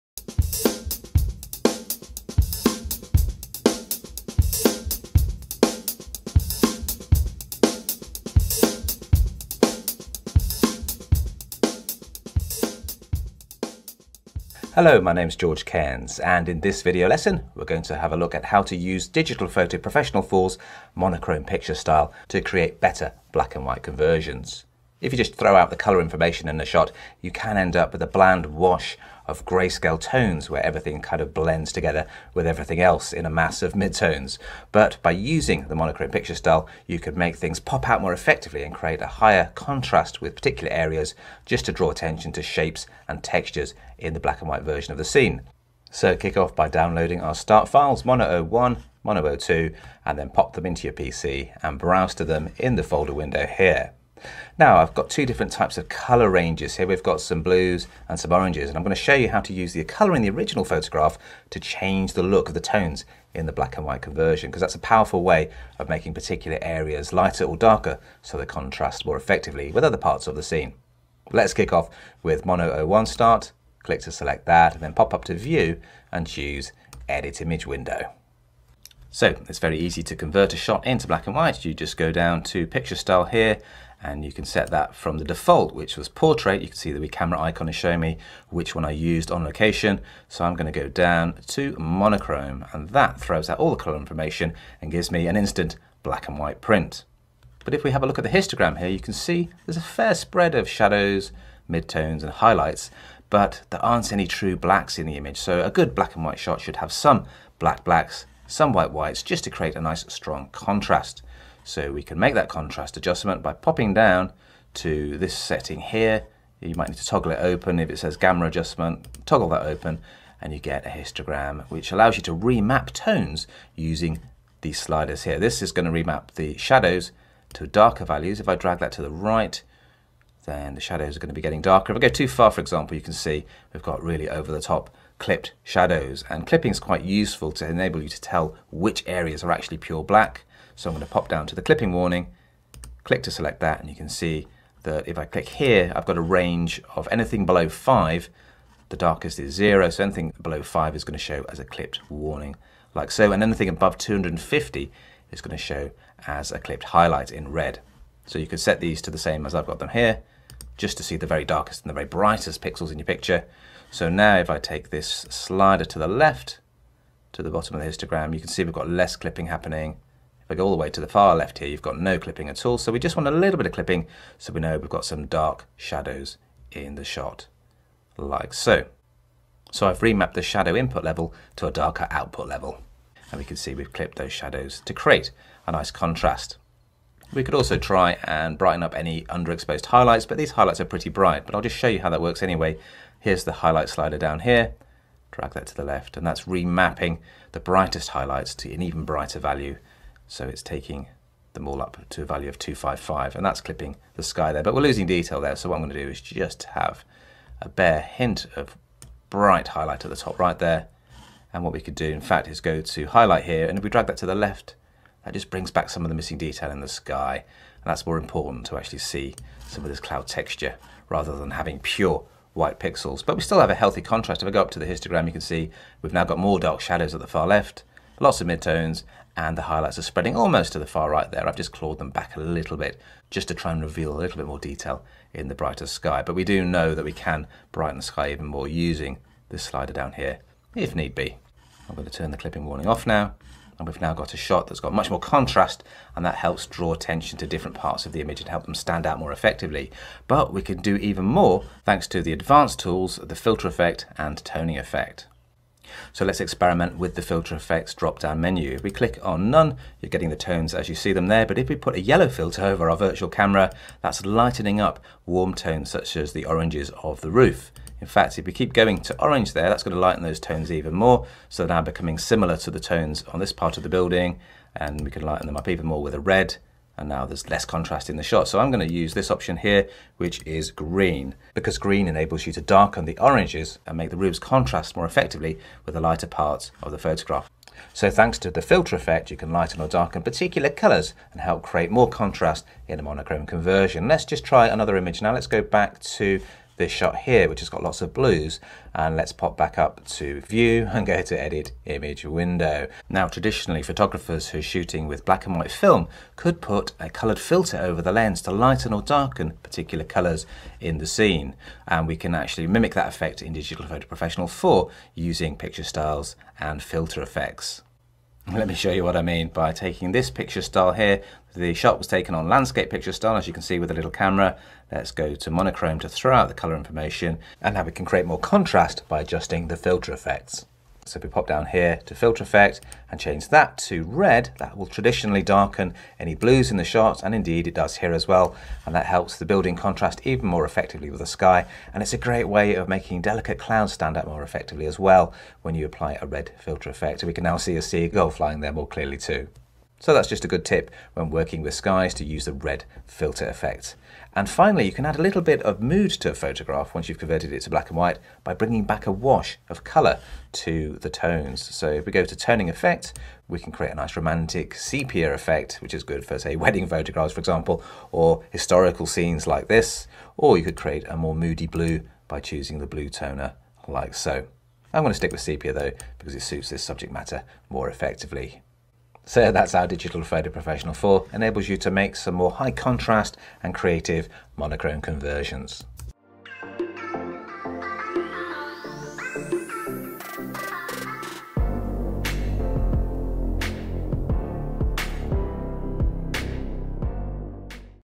9 Better Black & White Photos - Canon DPP 4
Create effective monochrome conversions with Digital Photo Professional 4. An effective black and white conversion should feature contrasting tones that help various objects, textures and patterns stand out more effectively. You'll learn how to use the histogram and clipping warnings and the selective tone tweaking sliders to create a stronger contrast with some black shadow and white highlights in the mono conversion.